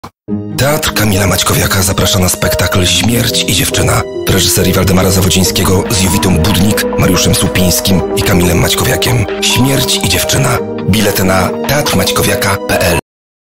Teatr Kamila Maćkowiaka - Spektakl "Śmierć i dziewczyna" [promo video]
"śmierć i dziewczyna" - ulubiony dramat widzów Teatru Kamila Maćkowiaka. W rolach głównych: Jowita Budnik, Mariusz Słupiński i Kamil Maćkowiak. Z okazji czwartej rocznicy premiery spektaklu, bilety dostępne z rabatem 40%. W repertuarze tylko w dniach 3 i 4 lutego 2024 r. Więcej na